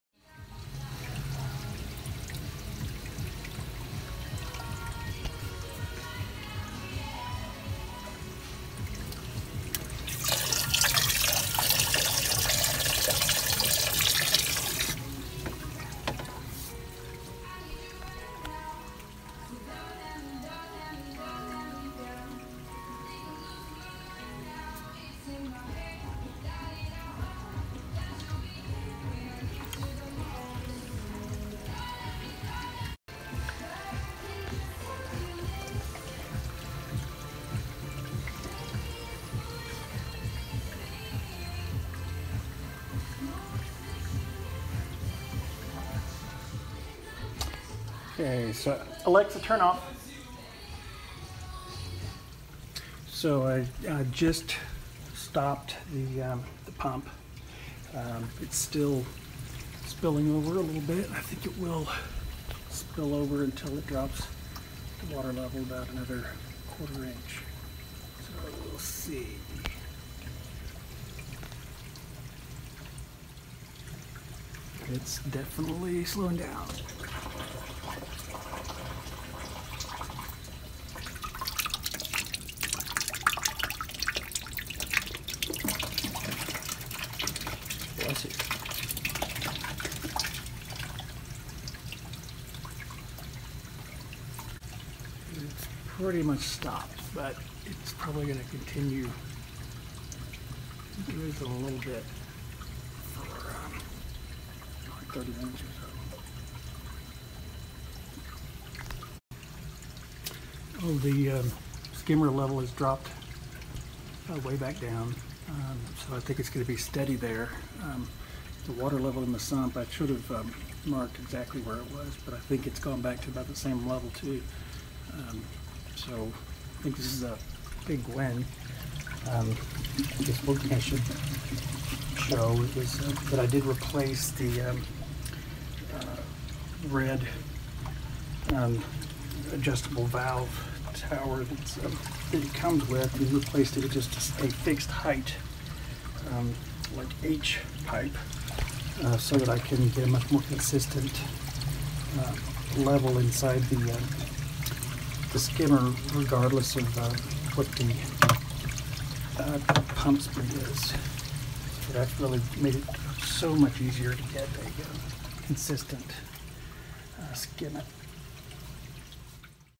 Skimmer-Assisted Water Change
This is my first test using a protein skimmer to extract waste water as new salt water is added